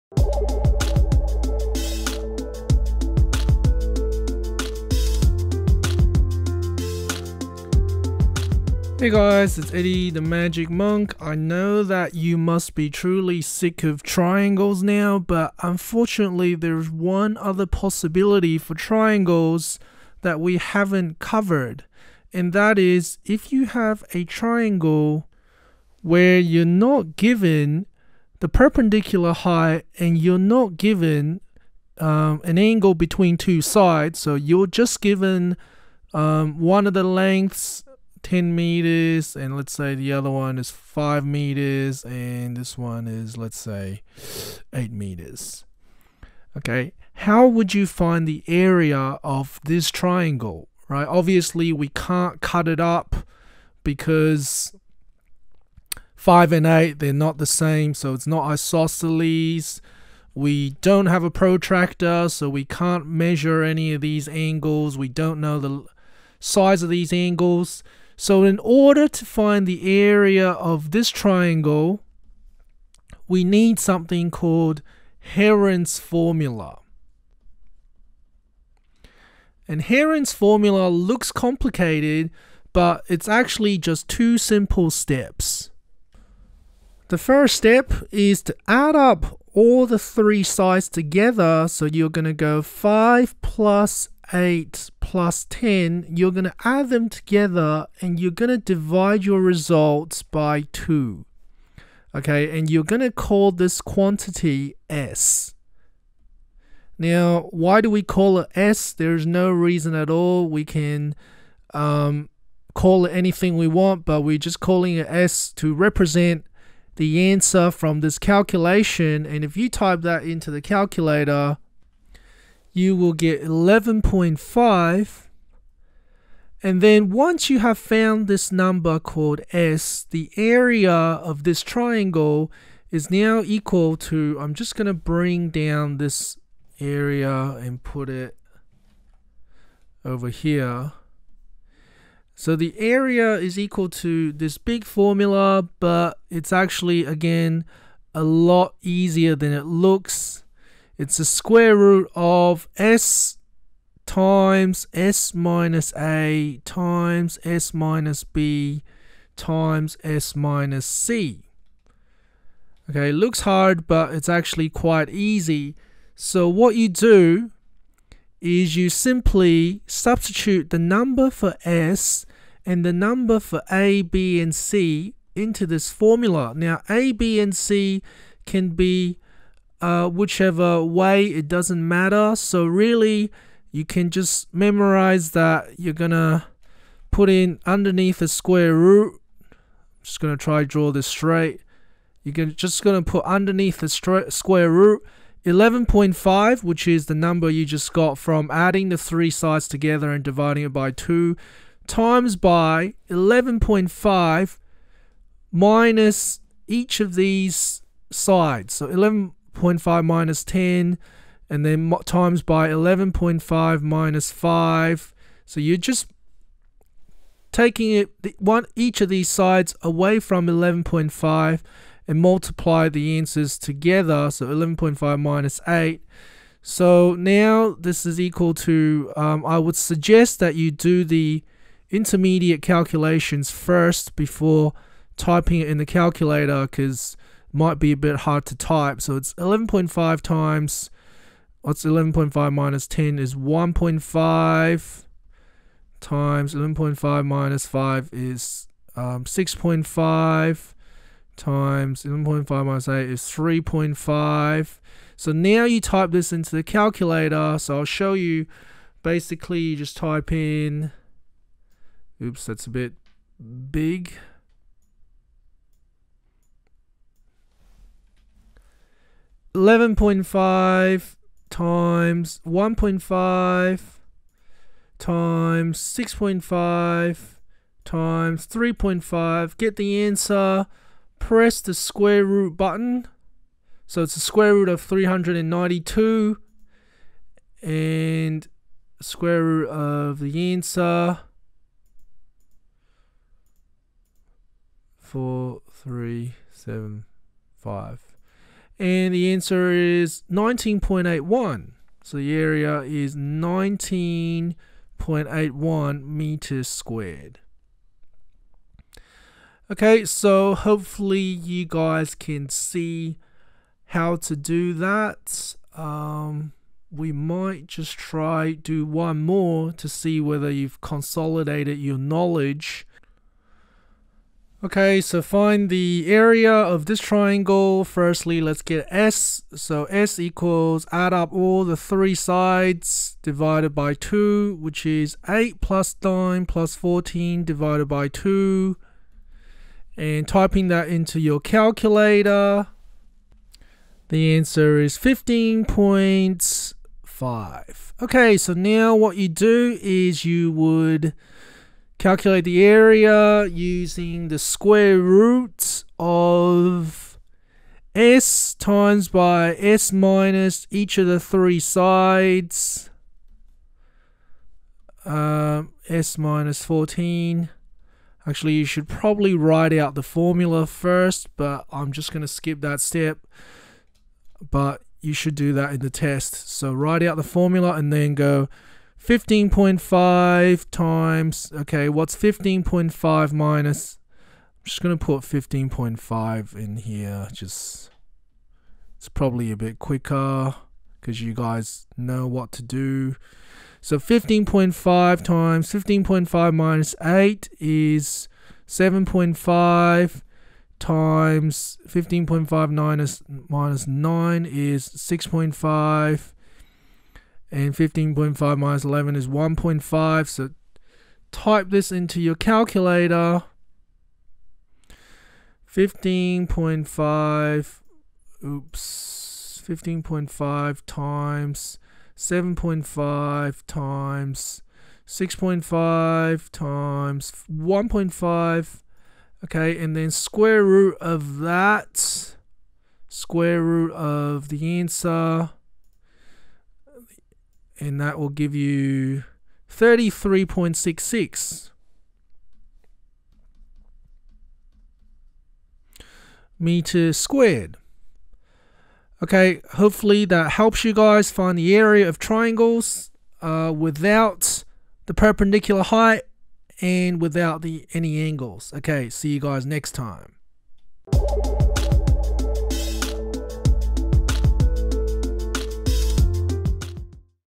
Surveying Maths A lesson 3 - Use Heron's formula to find the area of a triangle
In this series of videos, we will teach you the basic maths used by a surveyor. In this video we teach you how to find the area of a triangle given the lengths of all three sides, using Heron's formula.